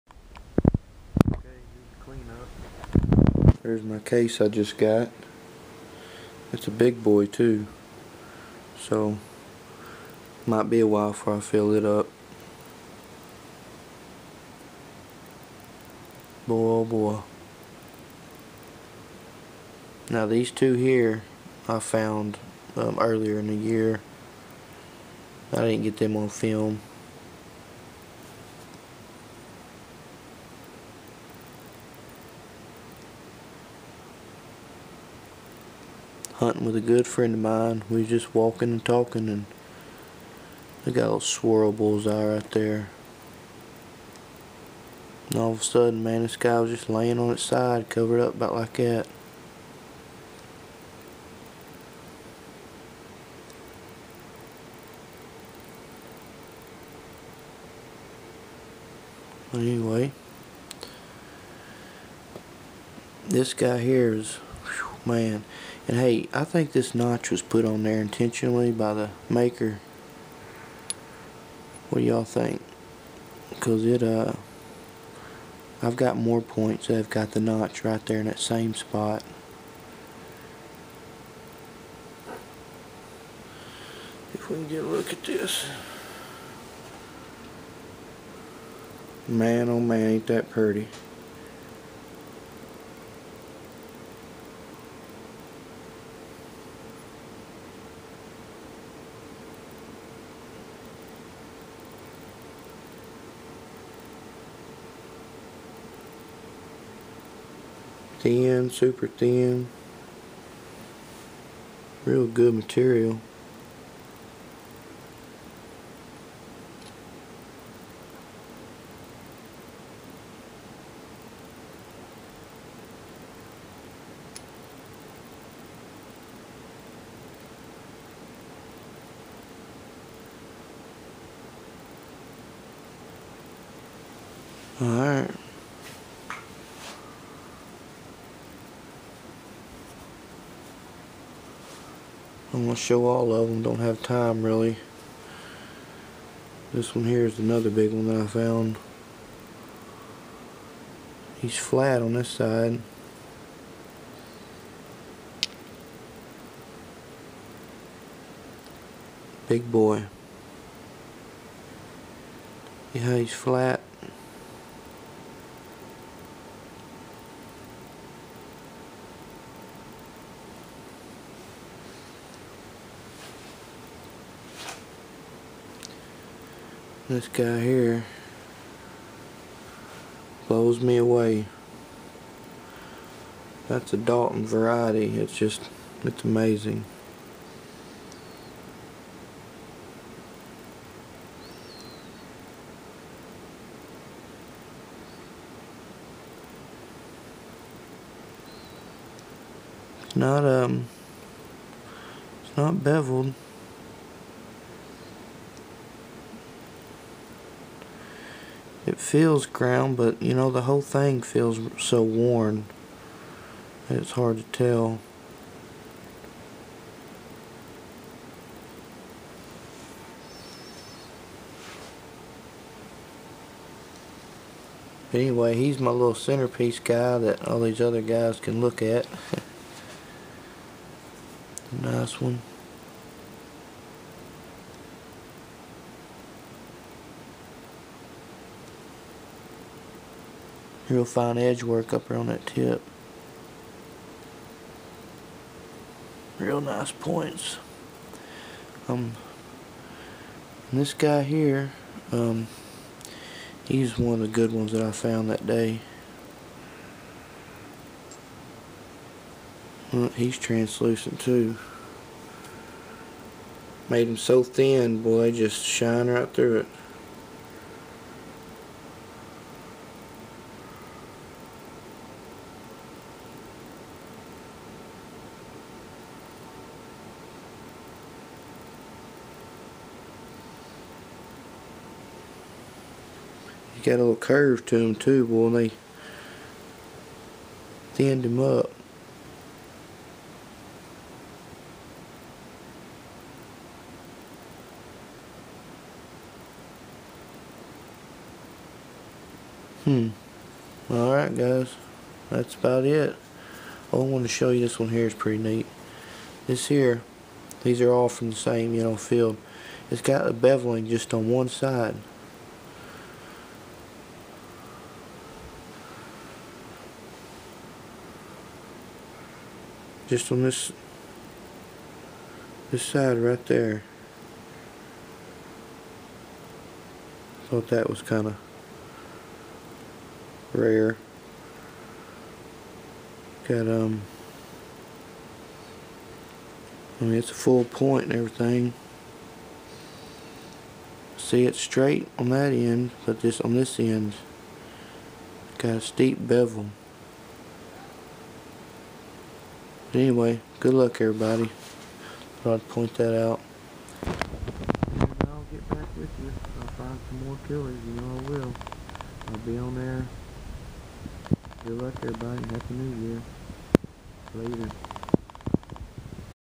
indian artifact site case clean up! cache site
well heres the clean up and my new site case! hope everybody has a great year comming up! thanks for comming along and i look forward  to seeing what else we will pull from the dirt!